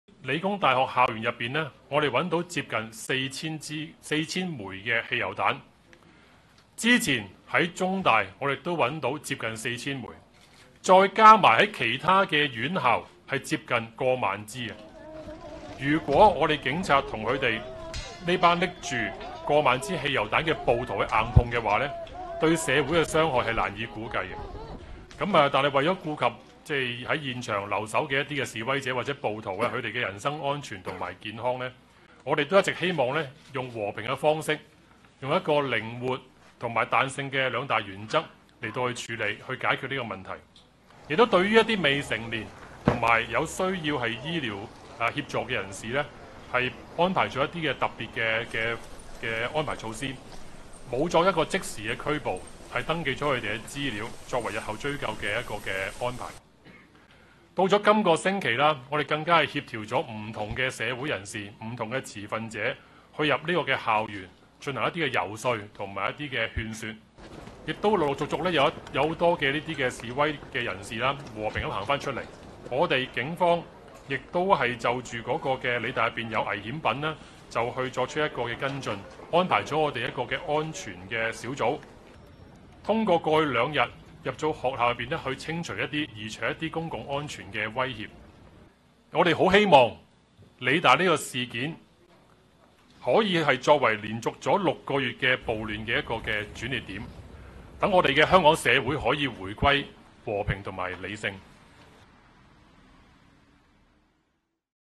Hong Kong police find almost 4,000 petrol bombs on PolyU campus
Almost 4,000 petrol bombs were found in two days while clearing the Hong Kong Polytechnic University campus, said Hong Kong police at a media briefing Friday.